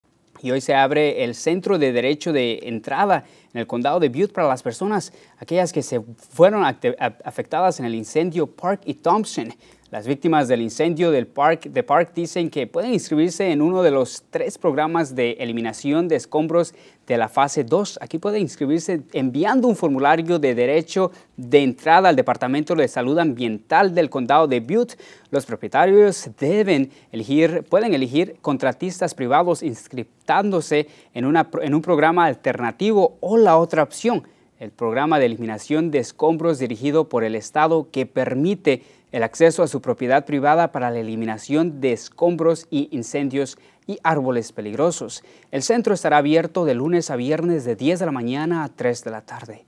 Abre el Centro de Derecho de Entrada del condado de Butte
Los propietarios no pueden realizar por sí mismos las limpiezas de la fase 2. El Centro ROE se encuentra en 765 East Avenue en Chico.